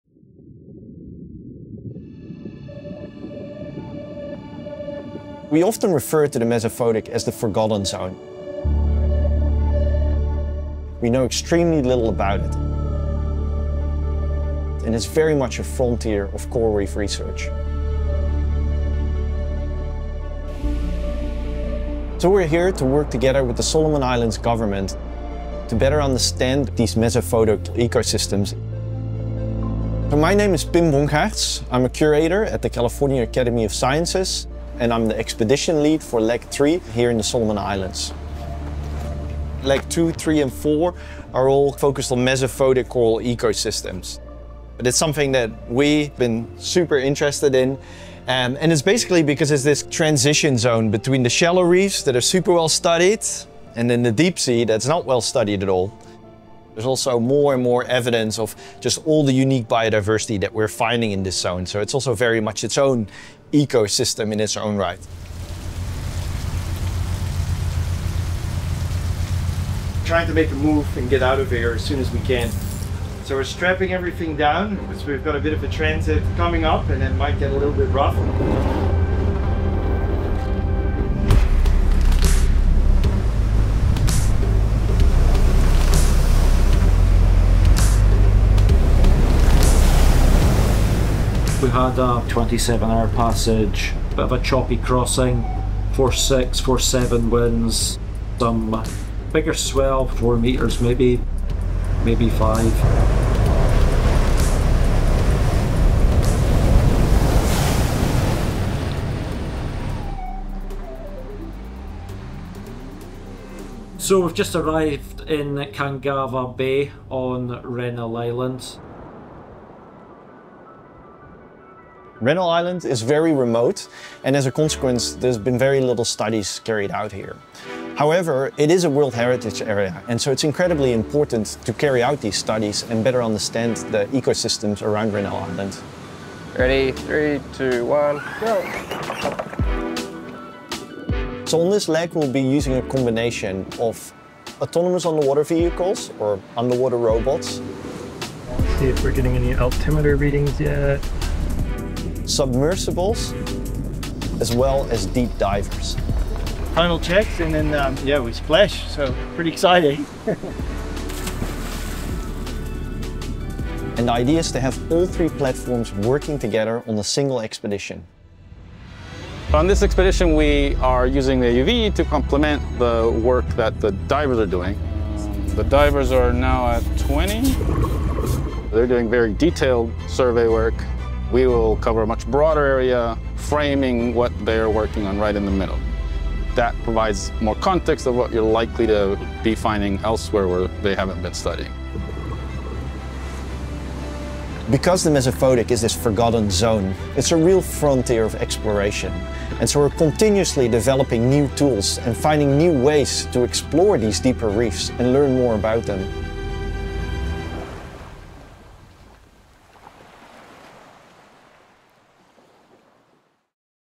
Expedition Solomon Islands – Leg 3 | Mesophotic Photogrammetary & Genetic Sampling
On the third leg of our journey, the Inkfish team's exploration of largely uncharted mesophotic reef ecosystems in the Solomon Islands continues with a new team of scientists focusing on the biology of these deep-water reefs.

Using a multi-platform approach, leg 3 utilises underwater autonomous vehicles to create large-scale photogrammetry maps of the reef, and rebreather divers to capture ultra-high resolution 3D models and conduct genetic sampling to describe the coral biodiversity.

The overall goal is to gain a better understanding of the composition and resilience of mesophotic coral ecosystems in the face of rapid ocean change.